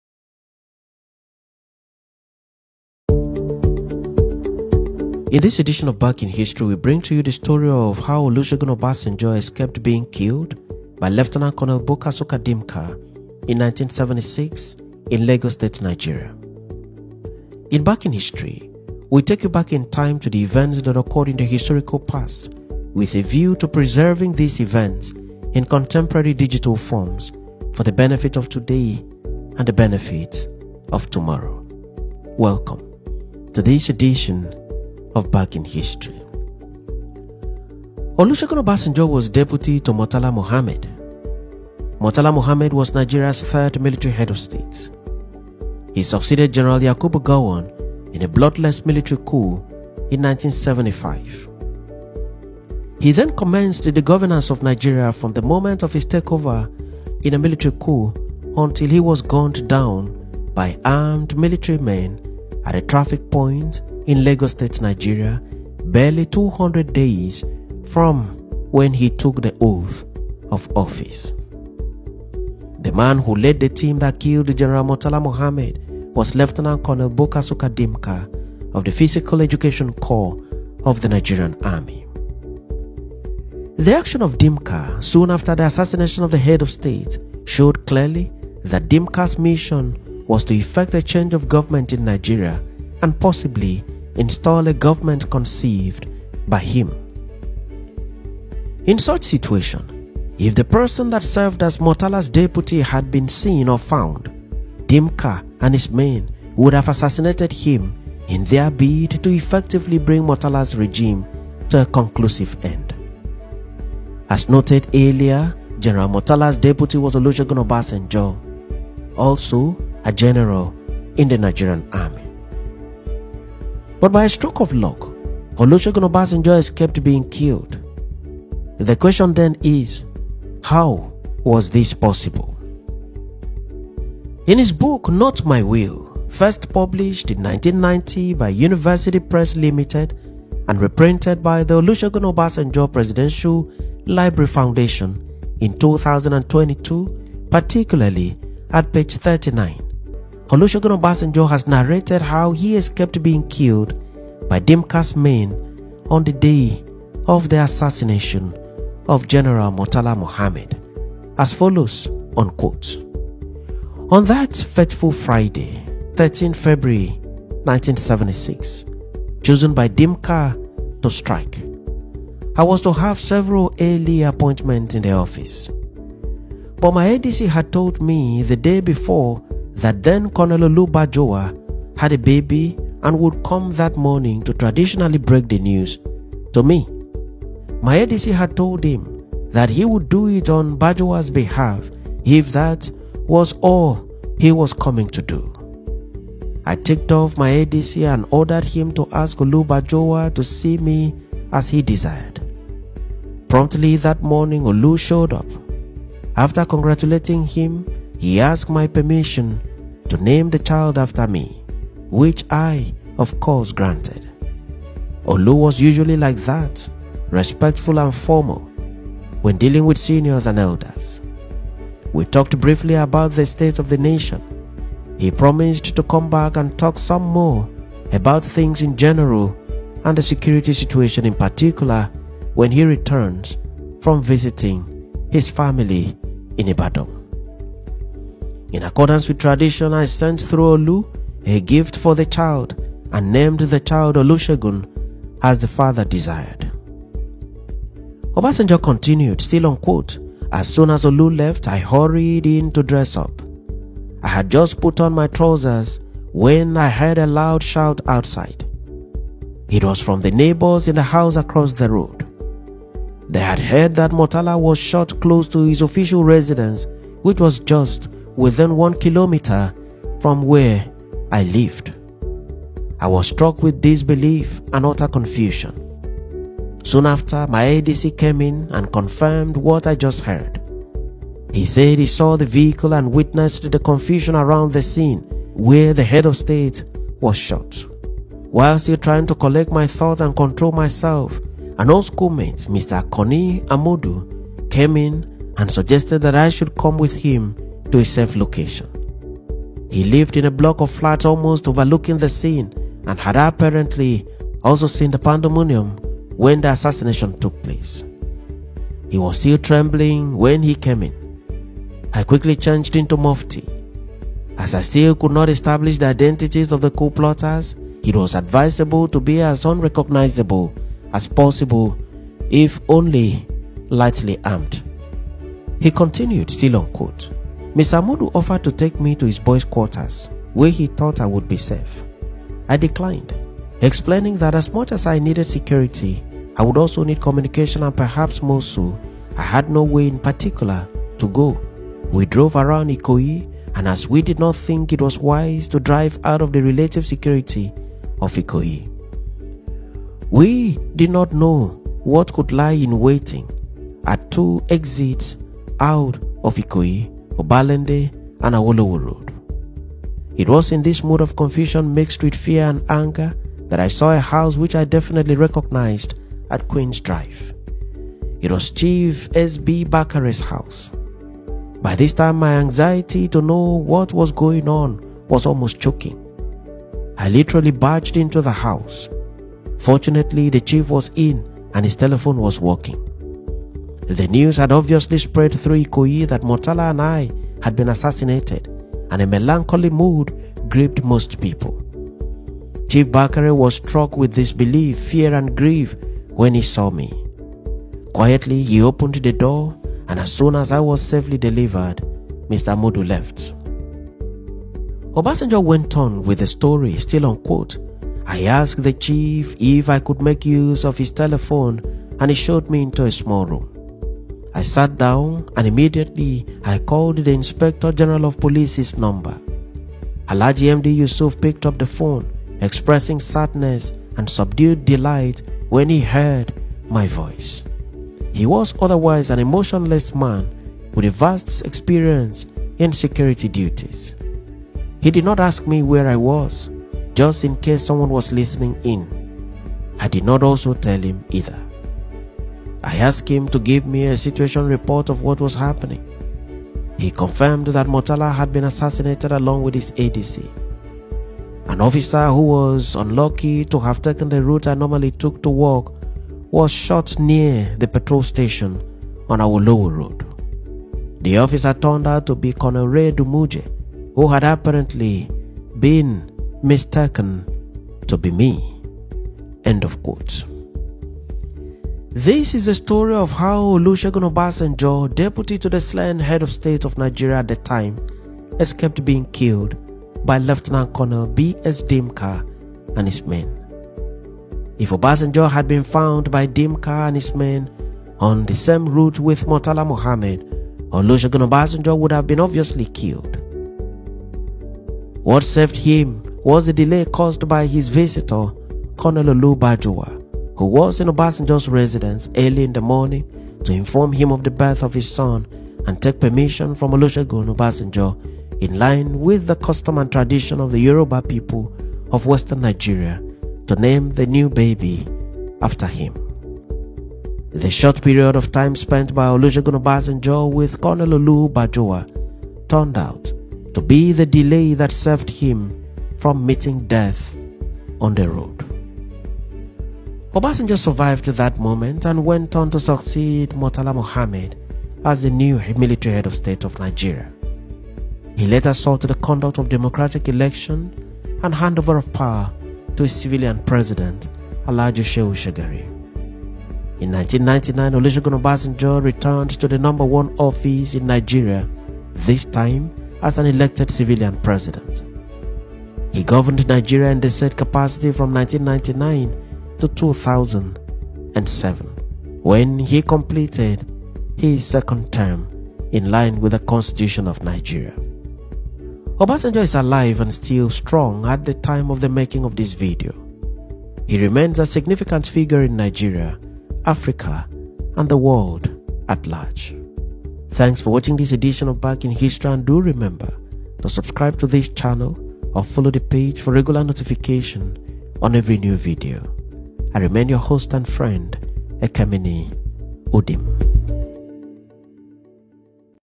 How Gen.Olusegun Obasanjo excaped being killed by Lt. Col Buka SukaDimka
How Gen.Olusegun Obasanjo excaped being killed by Lt. Col Buka Suka
Dimka